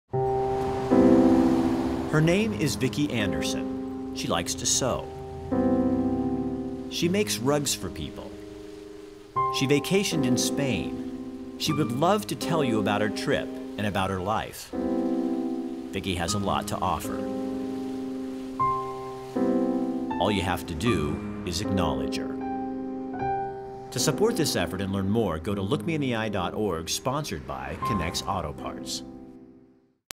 Look Me In The Eye: Bringing Value to People with Disabilies
Look Me In The Eye is an educational campaign to draw attention to the needs of people with disabilities. The idea of this promo is to show that by paying attention to people with disabilities not only can you enrich your life and the life of your community, you help these folks experience the value they so richly deserve.